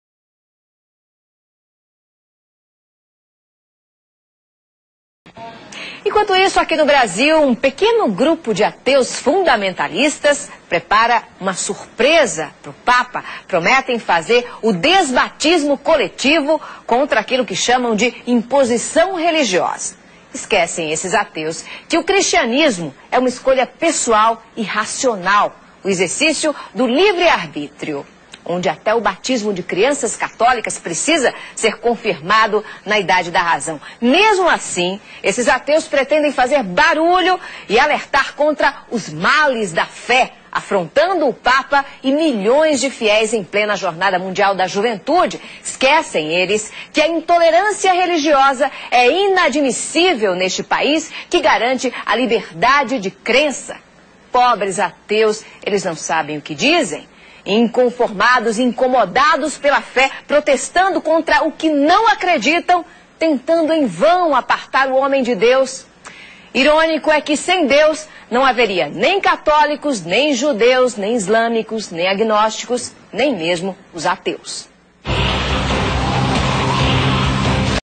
Brazilian TV Criticizes Atheists for protesting against the Pope
If you don't see the captions. Please turn it on on the cc icon below. May not work on all devices like phones, consoles and smart TVs.

Rachel Sheherazade is a TV host and pretty much like Fox News style she is very conservative and she has criticized the Brazilian atheist organization ATEA for promoting a protest upon the Pope's visit to Brazil for the World Youth Day. The Brazilian government has spent almost US$ 60 millions with the preparations and the Rio de Janeiro Mayor detected a two days holiday.